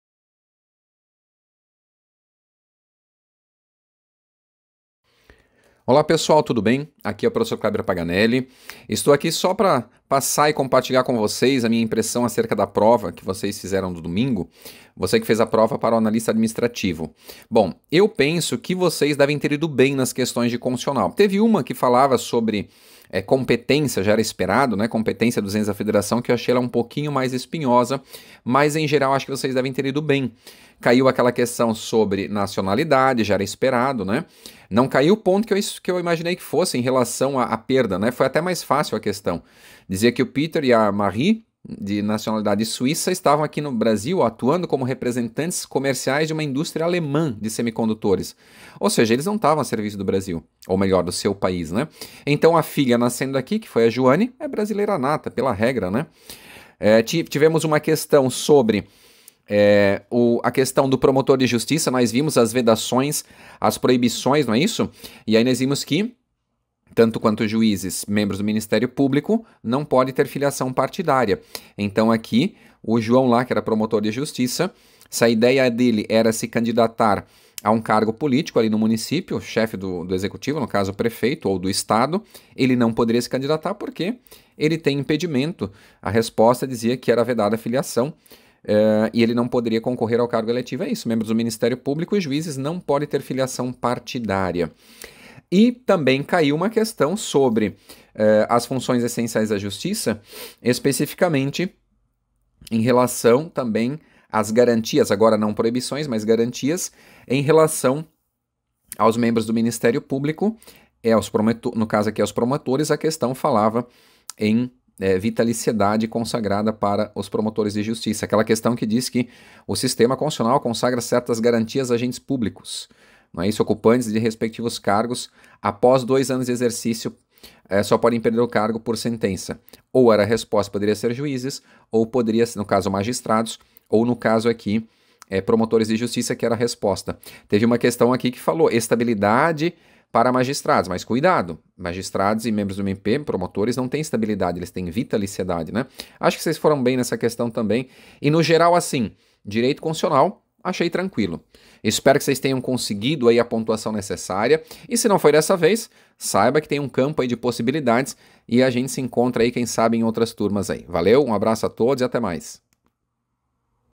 Prova Comentada TJSC A. ADMINISTRATIVO - Direito Constitucional
Nesse vídeo, o professor de Direito Constitucional do Rico Domingues, Cleber Paganelli,  fala sobre as questões da Prova para TJSC 2024.